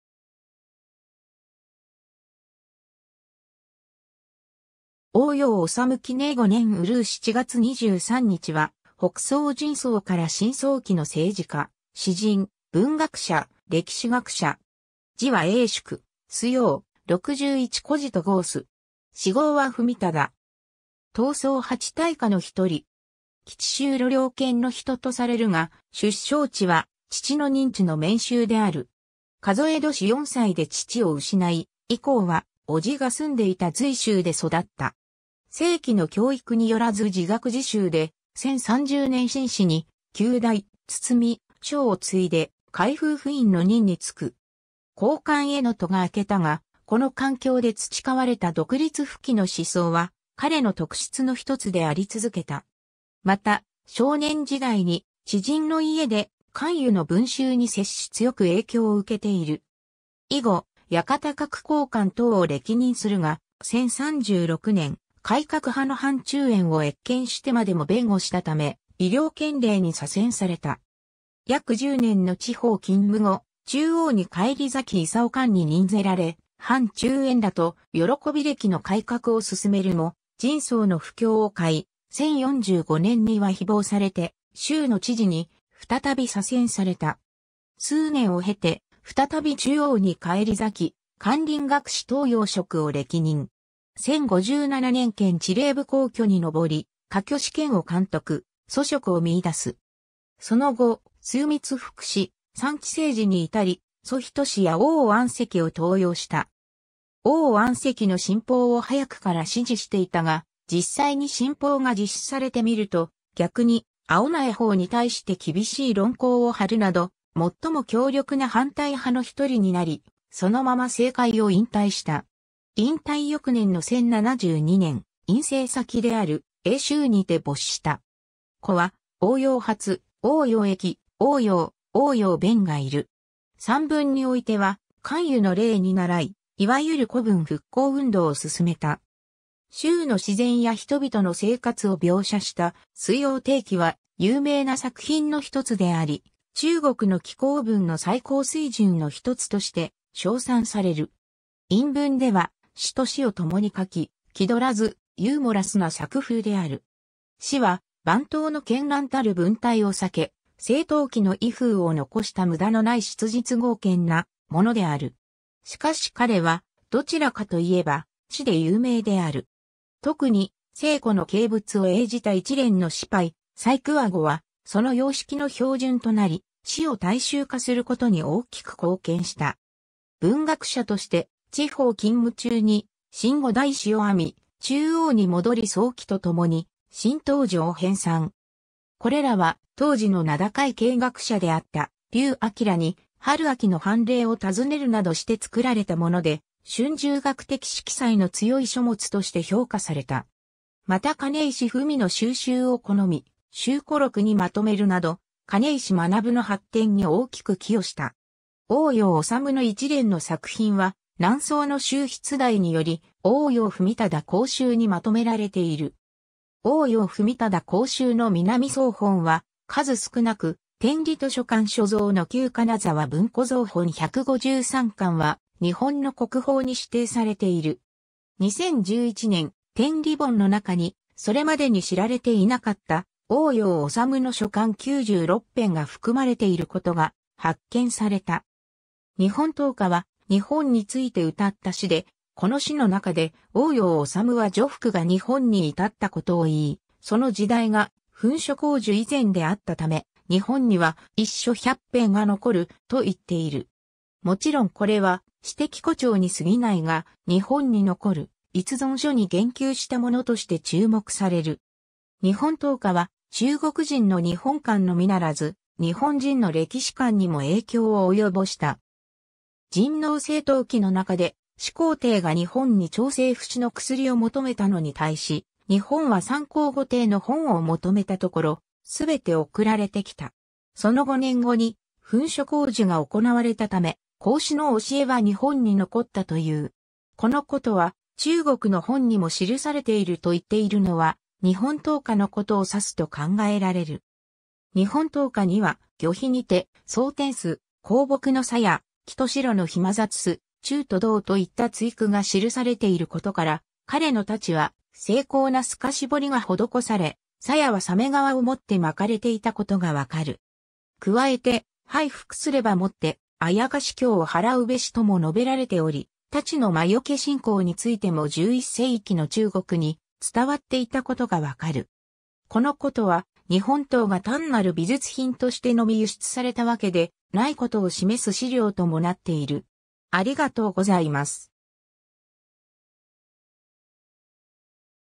欧陽脩
欧陽 脩（おうよう しゅう、欧陽修とも、景徳4年6月21日（1007年8月6日） - 熙寧5年閏7月23日（1072年9月8日））は、北宋仁宗から神宗期の政治家、詩人・文学者、歴史学者。
字は永叔、醉翁・六一居士と号す。
諡号は文忠。
唐宋八大家の一人。
 吉州廬陵県（現在の江西省吉安市）の人とされるが、出生地は父の任地の綿州（現在の四川省綿陽市）である。
数え年四歳で父を失い、以降は叔父が住んでいた随州で育った。
正規の教育によらず自学自習で、1030年（天聖8年）進士に及第、包拯を継いで、開封府尹の任に就く。
高官への途が開けたが、この環境で培われた独立不羈の思想は、彼の特質のひとつであり続けた。
また、少年時代に知人の家で韓愈の文集に接し強く影響を受けている。
以後、館閣校勘等を歴任するが、1036年（景祐3年）、改革派の范仲淹を越権してまでも弁護したため、夷陵県令に左遷された。
 約10年の地方勤務後、中央に返り咲き諌官に任ぜられ、范仲淹らと慶暦の改革を進めるも、仁宗の不興を買い、1045年（慶暦5年）には誹謗されて滁州（安徽省）の知事に再び左遷された。
 数年を経て、再び中央に返り咲き、翰林学士等要職を歴任。
1057年（嘉祐2年）権知礼部貢挙に上り、科挙試験を監督、蘇軾を見いだす。
その後、枢密副使・参知政事（副宰相）に至り、蘇洵や王安石を登用した。
王安石の新法を早くから支持していたが、実際に新法が実施されてみると、逆に青苗法に対してきびしい論考を張るなど、最も強力な反対派の1人になり、そのまま政界を引退した。
引退翌年の1072年（熙寧5年）、隠棲先である潁州（安徽省）にて没した。
子は欧陽発、欧陽奕、欧陽棐、欧陽辯がいる。
 散文においては韓愈の例に倣い、いわゆる古文復興運動をすすめた。
滁州の自然や人々の生活を描写した「醉翁亭記」は、有名な作品のひとつであり、中国の紀行文の最高水準のひとつとして賞賛される。
 韻文では詩（漢詩）と詞をともに書き、気取らず、ユーモラスな作風である。
詩は、晩唐の絢爛たる文体を避け、盛唐期の威風を残した無駄のない質実剛健なものである。
しかし彼は、どちらかと言えば詞で有名である。
特に西湖の景物を詠じた一連の詞牌『采桑子』は、その様式の標準となり詞を大衆化することに大きく貢献した。
 文学者として、地方勤務中に『新五代史』を編み、中央に戻り宋祁とともに『新唐書』を編纂。
これらは当時の名高い経学者であった劉敞に春秋の凡例を尋ねるなどして作られたもので、春秋学的色彩の強い書物として評価された（ただし清朝になり史実重視の立場が勃興すると、従来評価されていた文学的側面が逆に批判される様になった）。
また金石文の収集を好み、『集古録』にまとめるなど金石学の発展に大きく寄与した。
 欧陽脩の一連の作品は、南宋の周必大により『欧陽文忠公集』にまとめられている。
『欧陽文忠公集』の南宋本は数少なく、天理図書館所蔵の旧金沢文庫蔵本153巻は日本の国宝に指定されている。
 2011年、天理本の中にそれまでに知られていなかった欧陽脩の書簡96篇が含まれていることが発見された。
 『日本刀歌』は日本についてうたった詩で、この詩の中で欧陽脩は徐福が日本にいたったことをいい、その時代が焚書坑儒以前であったため、日本には「逸書百篇」（失われた『書経』の篇）が残る、といっている。
もちろんこれは詩的誇張にすぎないが、日本に残る佚存書に言及したものとして注目される。
 『日本刀歌』は中国人の日本観のみならず、日本人の歴史観にも影響を及ぼした。
『神皇正統記』の中で、「始皇帝が日本に長生不死の薬を求めたのに対し、日本は三皇五帝の本を求めたところ、すべて送られてきた。その5年後に焚書坑儒が行われたため、孔子の教えは日本に残ったという。この事は中国の本にも記されている」と言っているのは『日本刀歌』のことを指すと考えられる。
 『日本刀歌』には、「魚皮にて装貼（そうてん）す、香木の鞘、黄と白の閑雑（かんざつ）す、鍮（ちゅう）と銅」といった対句が記されていることから、彼の太刀は精巧な透かし彫りが施され、鞘は鮫皮をもって巻かれていたことがわかる。
加えて、「佩服（はいふく）すればもって妖凶をはらうべし」とも述べられており、太刀の魔除け信仰についても11世紀の中国に伝わっていたことがわかる。
このことは、日本刀が単なる...